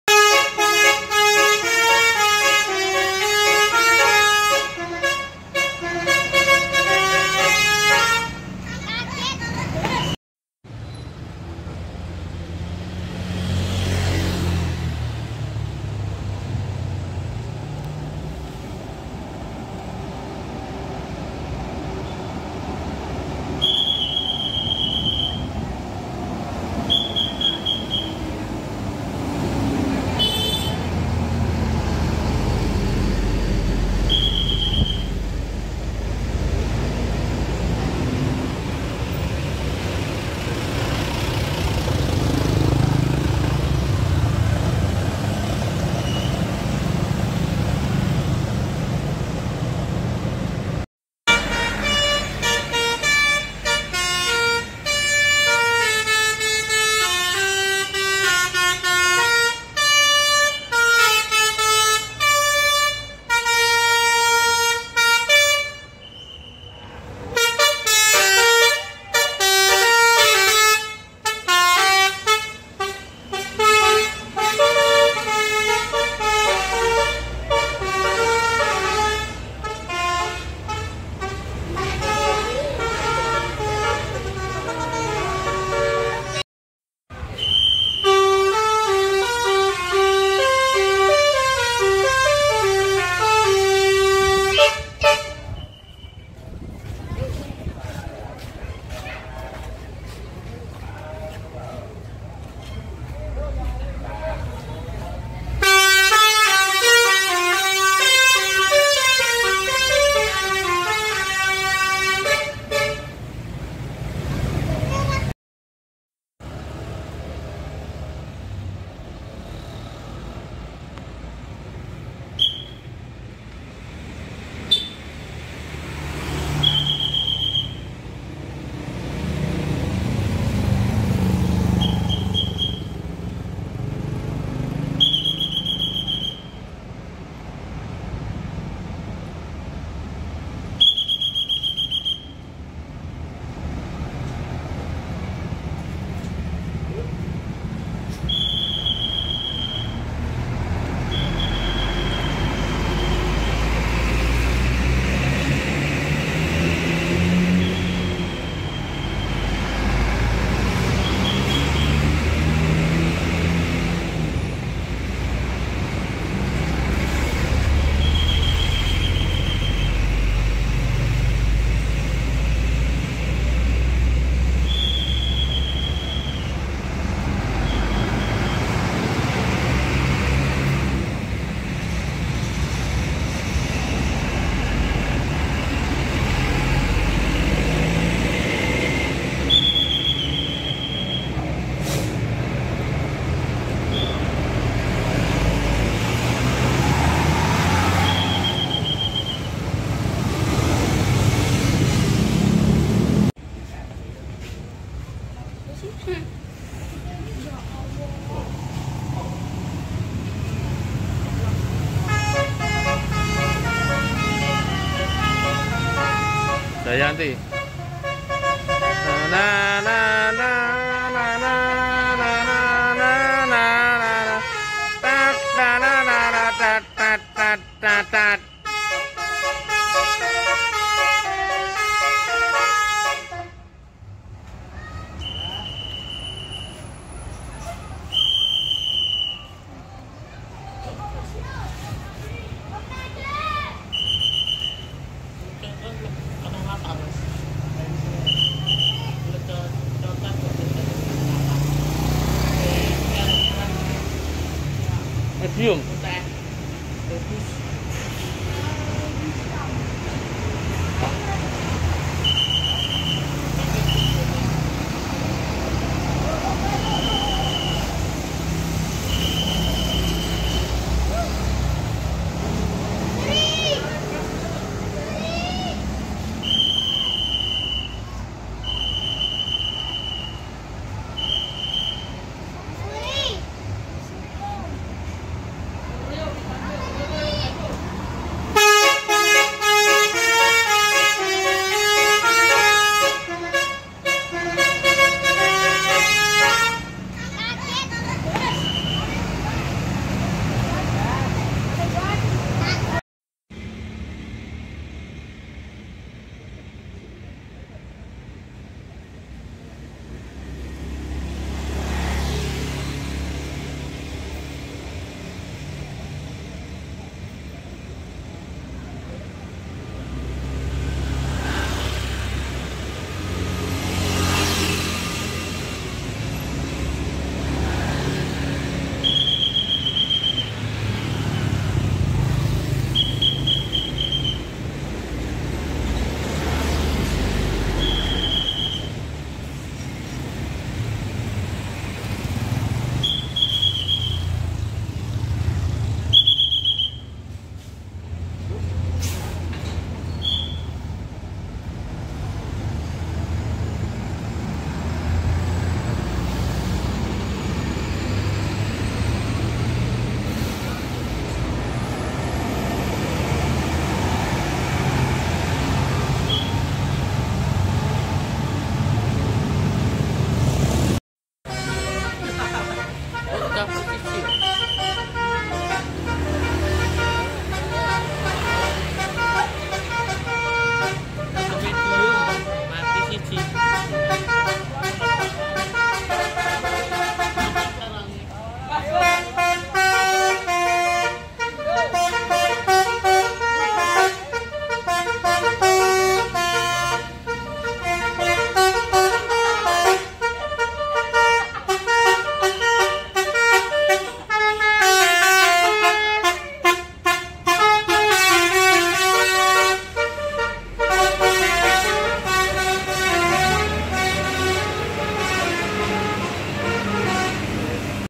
HUNTING BUS PARIWISATA OBAT KEGABUTAN
hallo teman teman pecinta bus dan basuri mania videp kali ini saya hunting bus pariwisata tanpa di temenin partner bocil mania.